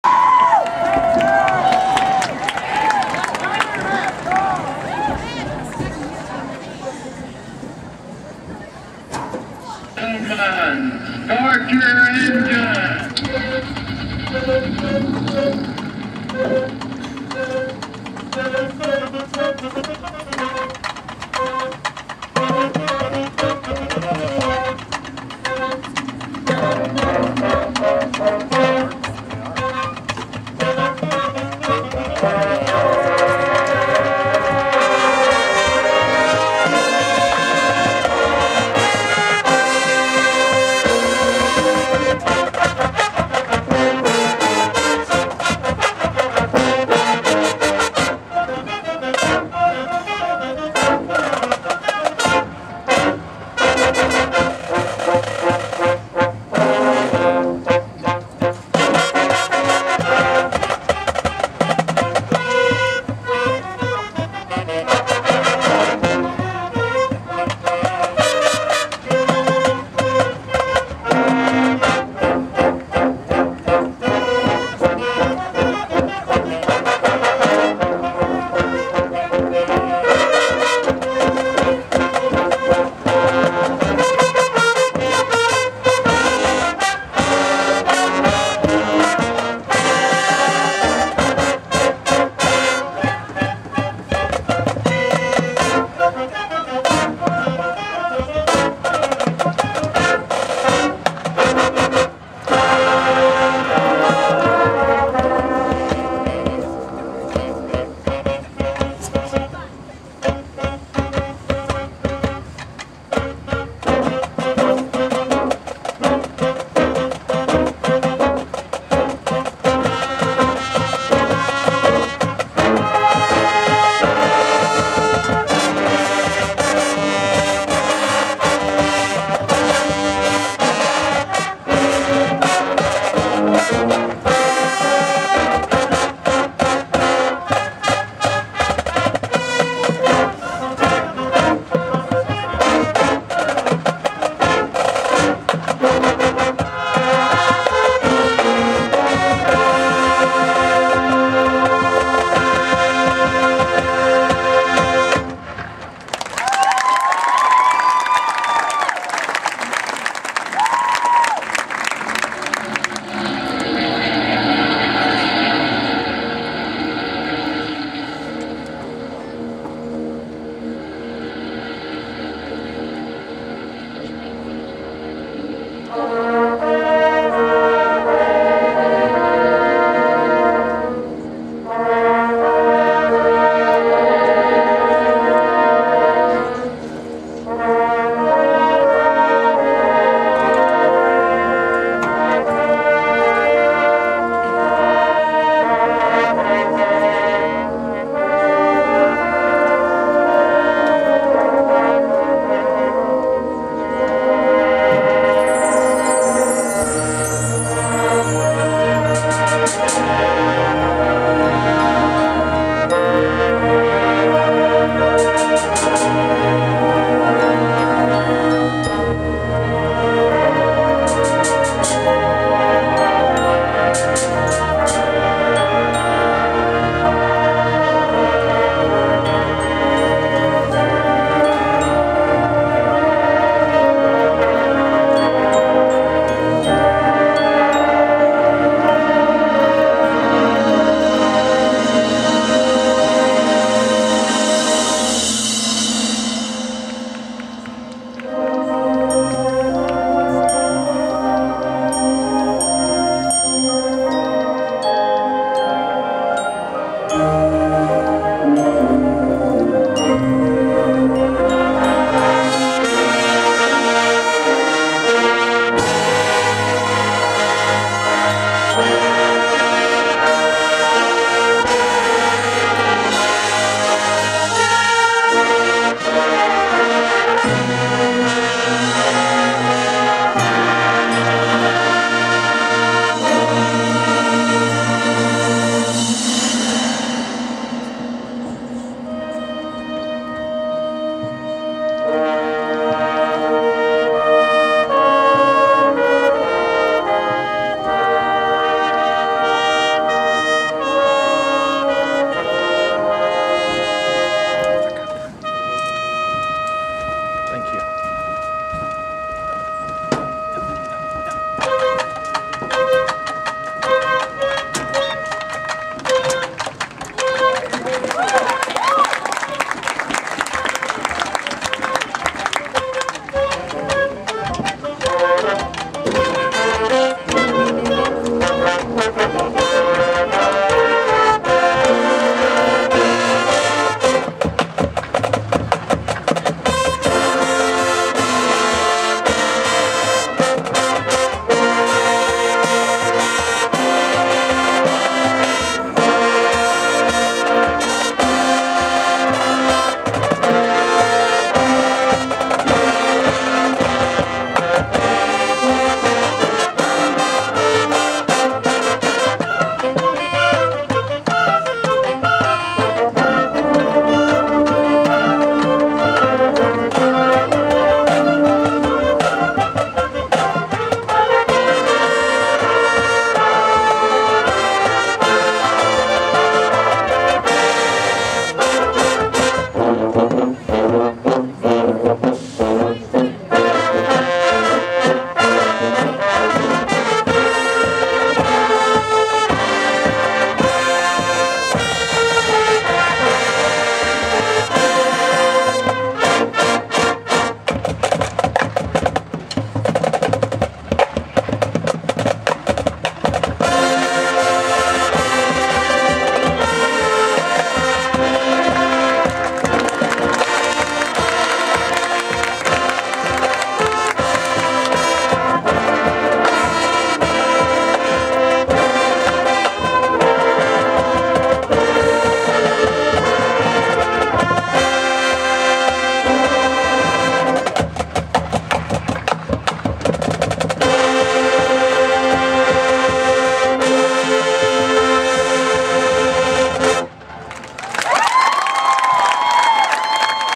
South Forsyth High School - Forsyth County Schools - Marching Band Exhibition
Forsyth County Schools - Marching Band Exhibition - South Forsyth High School on 10/11/2010 hosted by South Forsyth High School